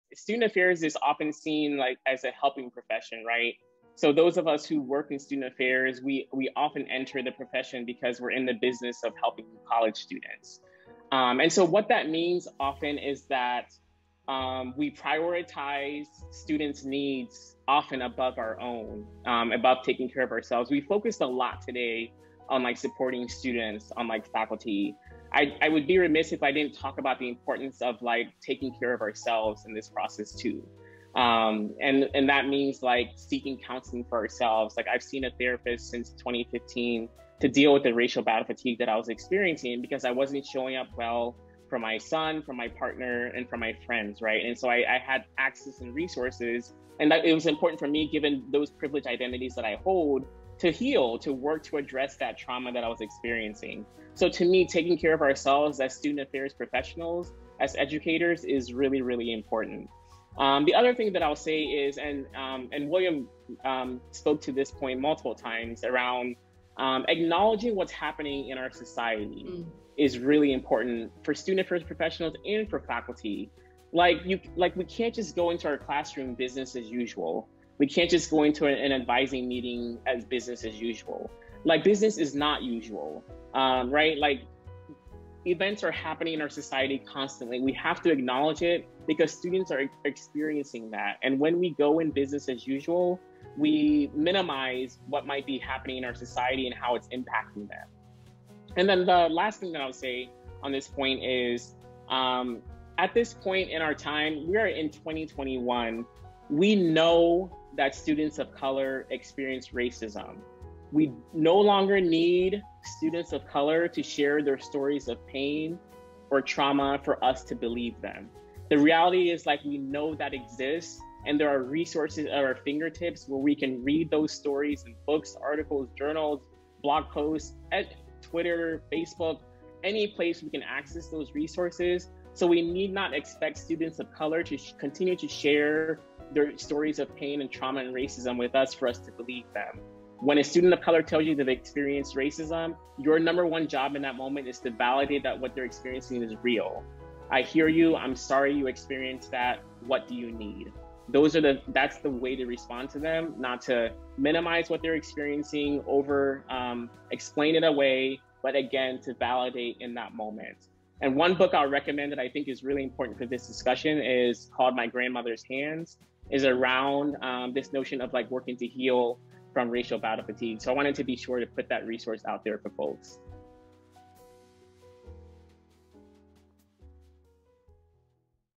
Returning to In-Person Instruction Webinar: Student Affairs, Stephen Quaye, PhD
Promoting Racial Equity in Student Mental Health:
Considerations and Strategies for Returning to In-Person Instruction

Welcome by Tabbye Chavous, PhD
Carlota Ocampo, PhD, Moderator
Panelists:
William D. Lopez, PhD, MPH
Stephen Quaye, PhD
Cirleen DeBlaere, PhD

Race & Equity Center, University of Southern California
The Steve Fund
National Center for Institutional Diversity, University of Michigan